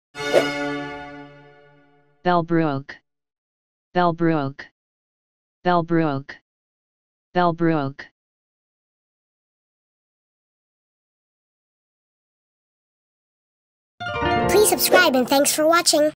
Bellbrook (How to Pronounce Cities of the World)💬⭐🌍✅
This video is about "Bellbrook". This video series is something special. We're covering the pronunciation of all the cities of the world.